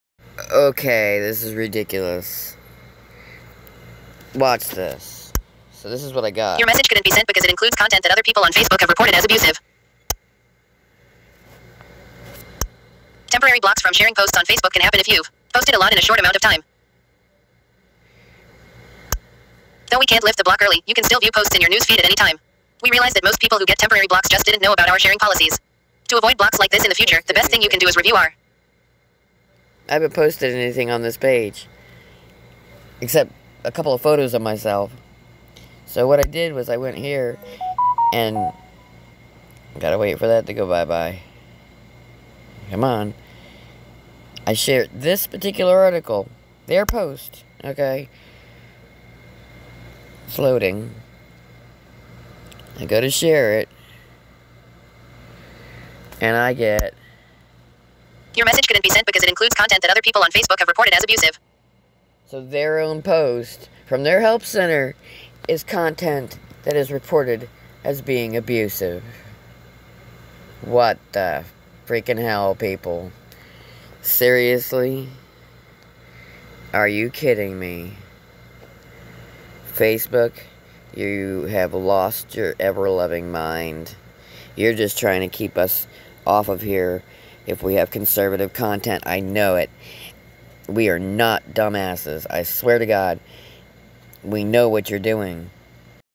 Can't post Facebook claims its own help center content has been reported as abusive
Over the last couple weeks I have it issues like crazy with Facebook 1st thing they did was take my account my boyfriend's account and to over adman's on our group and pages that we run the county down with no provocation no reason no notice just woke up one morning and we no longer have accounts then I use an account that I had sitting around that was with my soon to be hen name for my book that I'm writing and they tell me I can't use my pen name and I've gotta put my real name on there now how do they know this is my pen name what are they doing that lets them know this. So I go when I make an account that has an assimilation of my middle and last name which is perfectly legal as using my legal name and I don't post anything on it except some photos of myself so my friends when I add them know who the heck I am I go to look at it's A-day and like a post on somebody's account there was in my news feed and I'm told that my action goes against community standards in other words a metal like I'm not allowed to like anything it goes against community standards then I go to share the help center that their post their own post and it says it goes against community standards because it's been reported to go against community standards from their own help center Facebook is trying to do everything they can to keep us off of their platform before the election